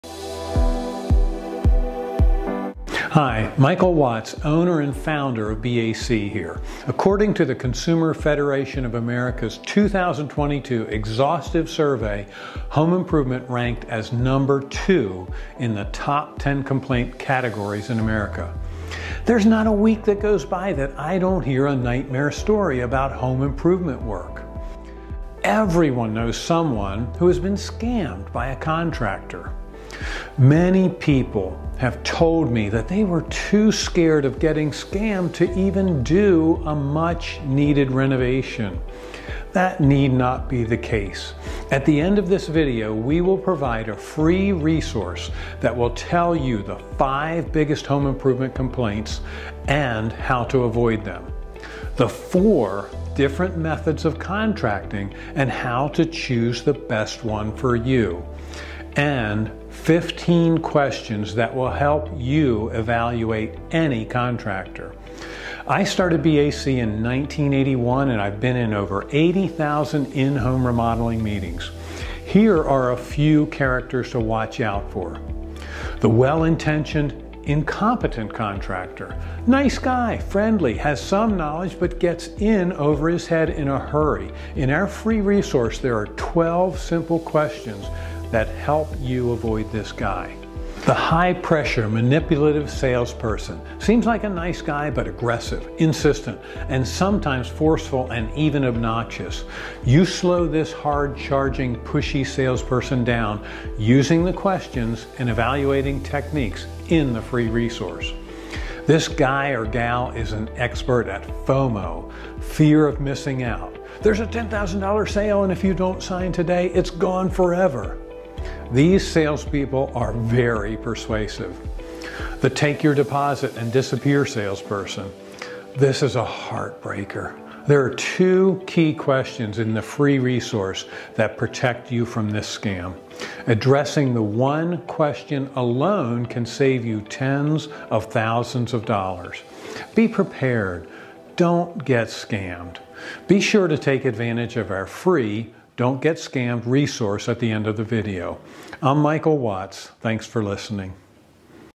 Guide to avoiding remodeling scams preview
Everyone knows someone who has been scammed by a contractor. Some people avoid needed renovations out of fear of being scammed. That need not be the case. Be prepared and you won’t get scammed. Watch this video by BAC president and owner Michael Watts to get a preview of our guide to avoiding remodeling scams.